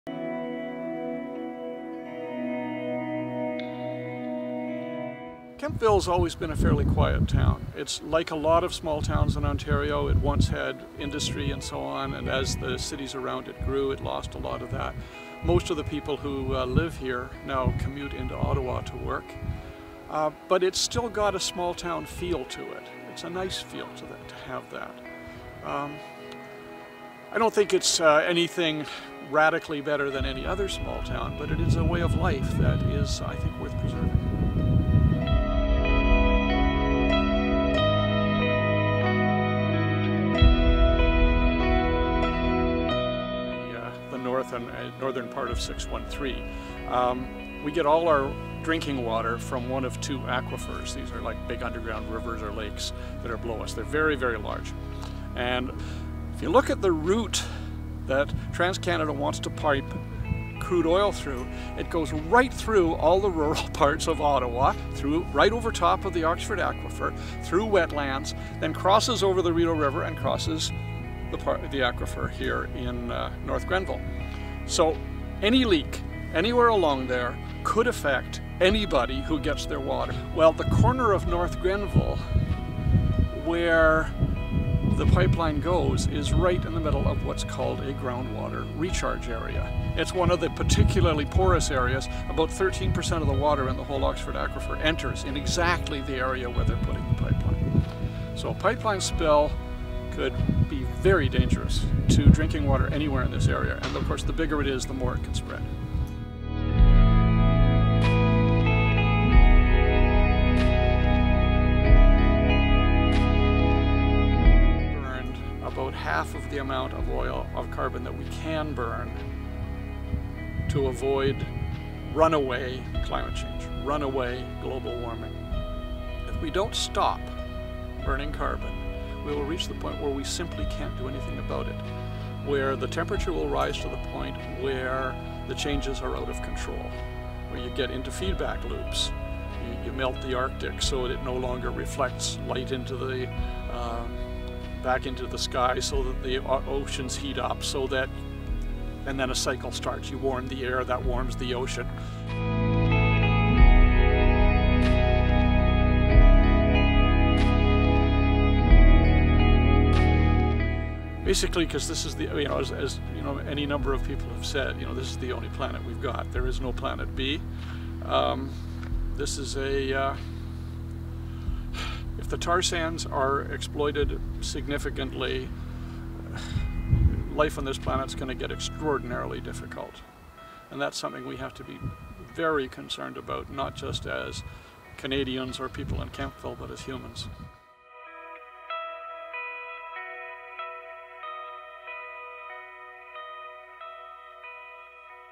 How Tar-sands Pipeline Will Impact Kemptville Community?
Interviewing Ian Angus about how tar-sands pipeline will impact Kemptville community, Ontario

Camera: Kathleen Black, Ben Powless
Video Editing: Wesley Fernandes de Sousa